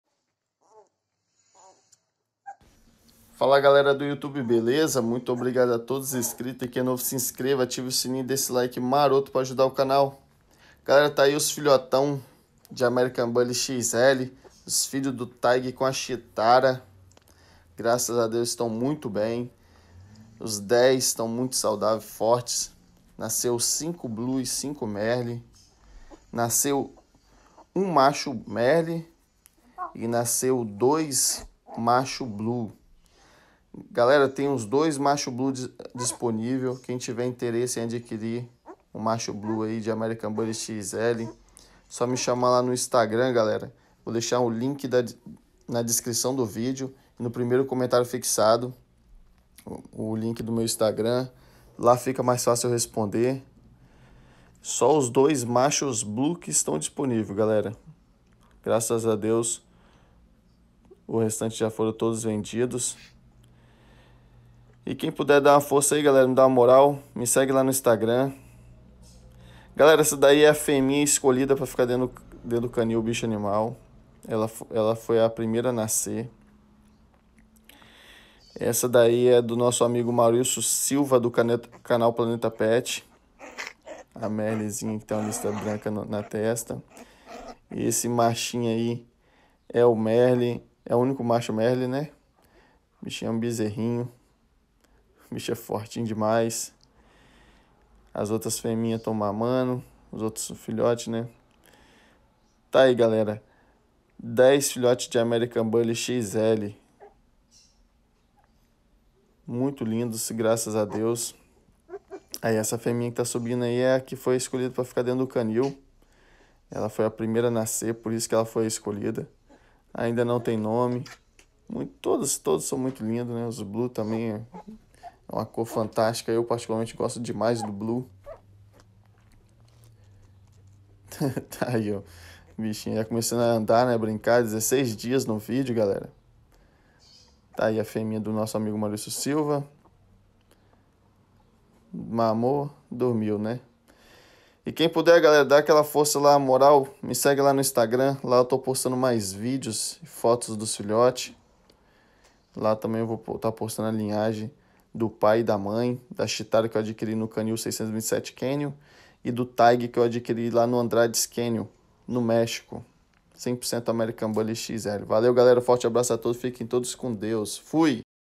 Mostrando os filhotes do ANDRADES TIGER X CHITARA , 16 Dias CANIL BICHO ANIMAL 🇧🇷
Obrigado galera